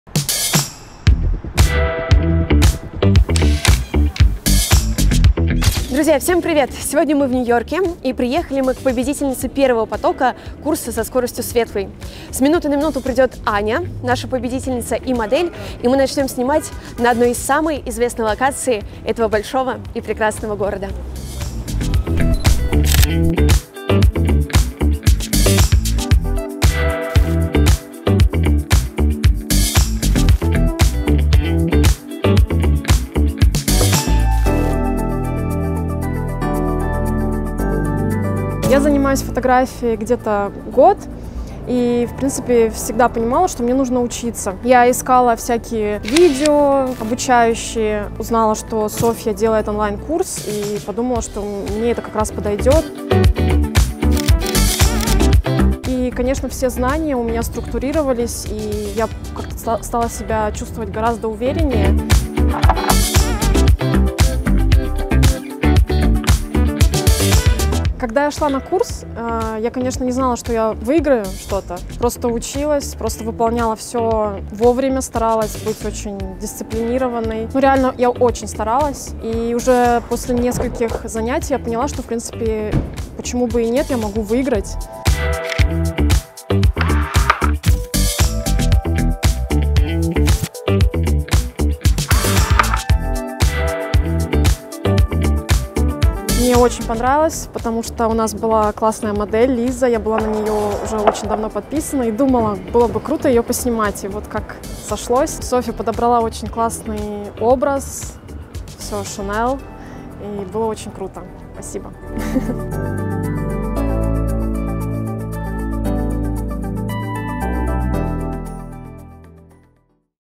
CHANEL - Behind the scenes - New York Photoshoot
Famous photographer from Russia did a photoshoot for Chanel in New York city. This is Behind the scenes of that shoot.

P.S. Not an official Chanel photoshoot campaign their clothes has been used for this shoot.
Color graded with : Filmconvert
Camera : Sonya7iii + Sony 24-70 GM